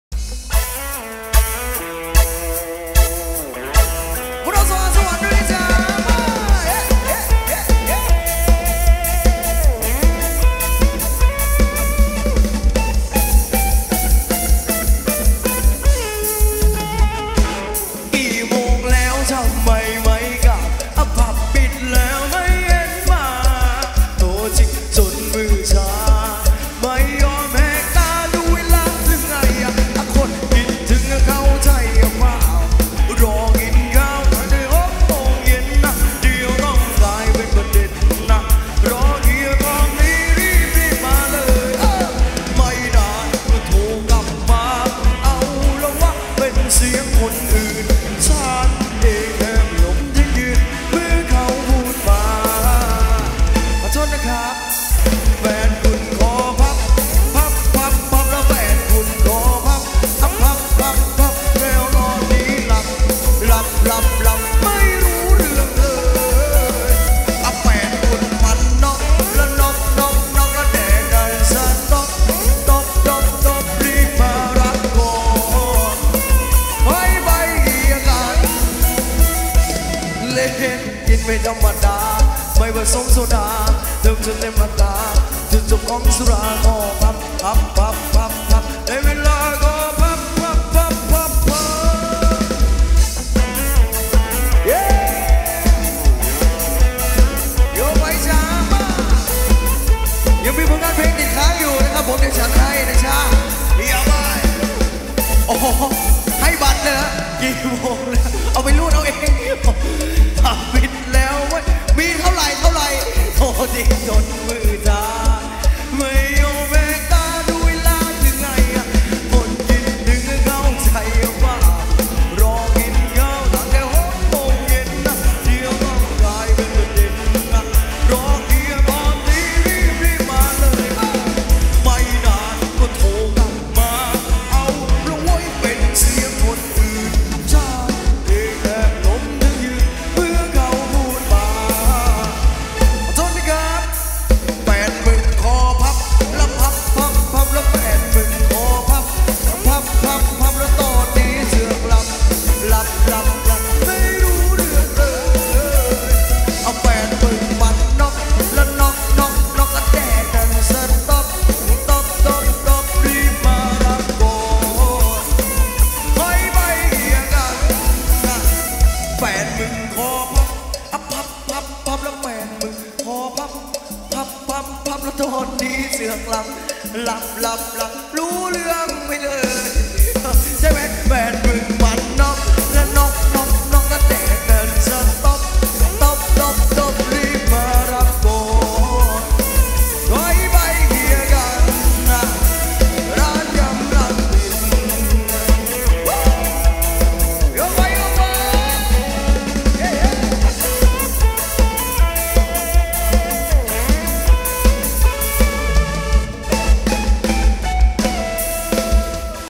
คอพับ😊รำวงเพชรบุรี ช่วงเพลงร่วมสมัยสนุกๆ😊ณ วัดพิกุลแก้ว😊วงดนตรีส_หาดสวรรค์😊น้องๆนางรำสาวสวย1คณะ
คอพับ😊รำวงเพชรบุรี ช่วงเพลงร่วมสมัยสนุกๆ😊ณ วัดพิกุลแก้ว😊
วงดนตรีส_หาดสวรรค์😊
น้องๆนางรำสาวสวย1คณะ
แสง เวที
โปรดทราบ...บนเวที เป็นพื้นที่ถ่ายภาพและวีดีโอ กล้องเยอะ ใครติดภาพ หรือวีดีโอ ไป ถือว่าช่างภาพไม่มีความผิด 😁